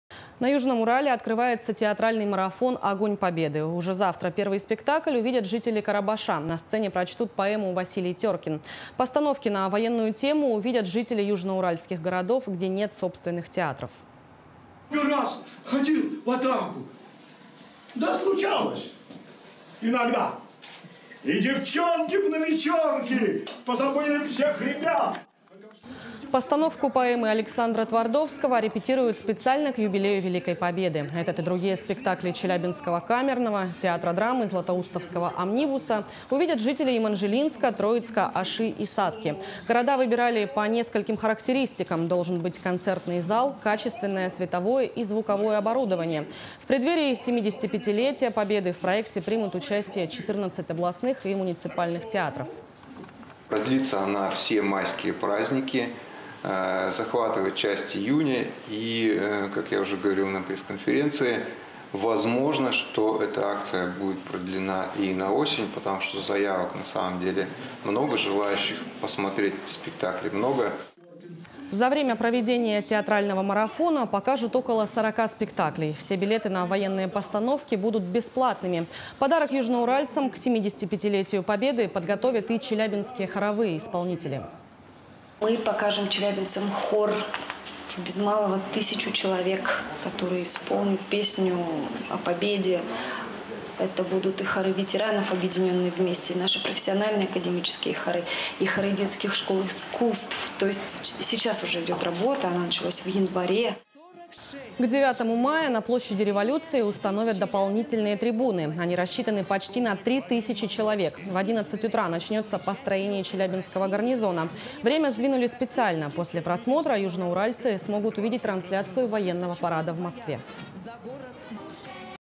На Южном Урале стартует театральный марафон
На Южном Урале открывается театральный марафон «Огонь Победы». Уже завтра первый спектакль увидят жители Карабаша — на сцене прочтут поэму «Василий Тёркин». Постановки на военную тему увидят жители южноуральских городов, где нет собственных театров.

Постановку поэмы Александра Твардовского репетируют специально к юбилею Великой Победы. Этот и другие спектакли челябинского Камерного, театра драмы, златоустовского «Омнибуса» увидят жители Еманжелинска, Троицка, Аши и Сатки. Города выбирали по нескольким характеристикам — должен быть концертный зал, качественное световое и звуковое оборудование. В преддверии 75-летия Победы в проекте примут участие 14 областных и муниципальных театров.

За время проведения театрального марафона покажут около 40 спектаклей. Все билеты на военные постановки будут бесплатными. Подарок южноуральцам к 75-летию Победы подготовят и челябинские хоровые исполнители.

К 9 мая на площади Революции установят дополнительные трибуны. Они рассчитаны почти на 3 тысячи человек. В 11 утра начнется построение челябинского гарнизона. Время сдвинули специально — после просмотра, южноуральцы смогут увидеть трансляцию военного парада в Москве.